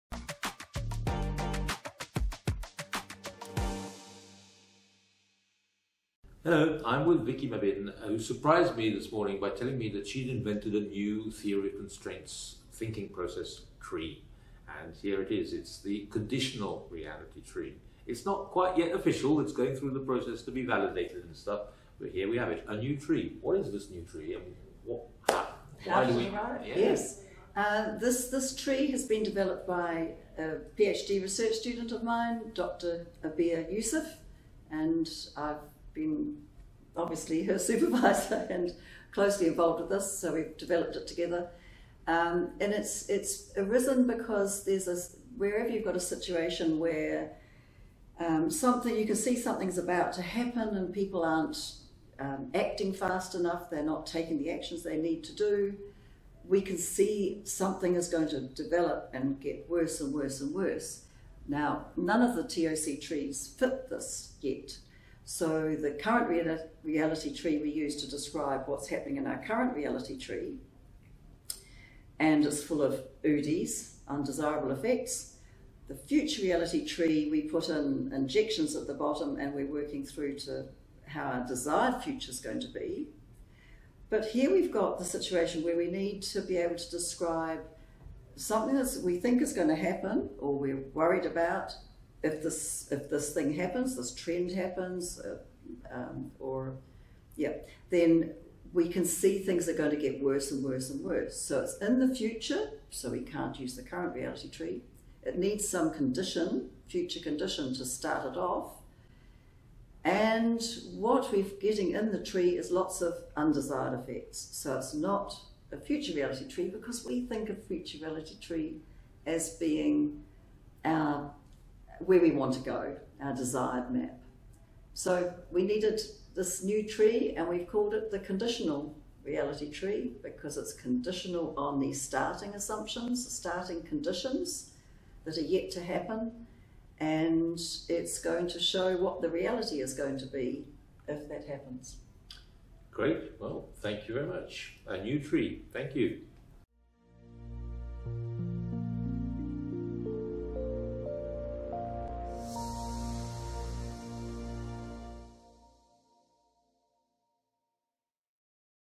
The Conditional Reality Tree by Vicky Mabin - Expert talk
In this video, Vicky Mabin talks about a new Theory of Constraints thinking process tree that she has invented with a student. It is called the Conditional Reality Tree. It is not quite official yet; it is still going through the validation process. This new tree has been developed by a PhD research student of Vicky, Dr. Abeer Youssef. She has been her supervisor and closely involved in the development process.

The Conditional Reality Tree has arisen because in situations where something is about to happen, and people are not acting fast enough or taking the necessary actions, we need a way to describe what might develop and worsen over time. None of the Theory of Constraints (TOC) trees currently fit this scenario. The Current Reality Tree (CRT) is used to describe the present situation, full of undesirable effects. The Future Reality Tree (FRT) involves injecting changes at the bottom to work towards our desired future. However, there are situations where we need to describe something that we think will happen or are worried about if a particular trend occurs. In this case, we can see that things will deteriorate in the future. Since it is about the future, we cannot use the Current Reality Tree.

The Conditional Reality Tree requires some future conditions to start it off. It includes lots of undesired effects, so it is not a Future Reality Tree, which typically represents our desired path. That is why we needed this new tree, and they have named it the Conditional Reality Tree because it is based on starting assumptions or conditions that are yet to happen. It will show what the reality is likely to be if those conditions occur.

This video was recorded in the United States in Hollywood Florida in 2023 at the TOCICO conference.

Marris Consulting is a management consulting firm.
Our motto: Factories, People & Results.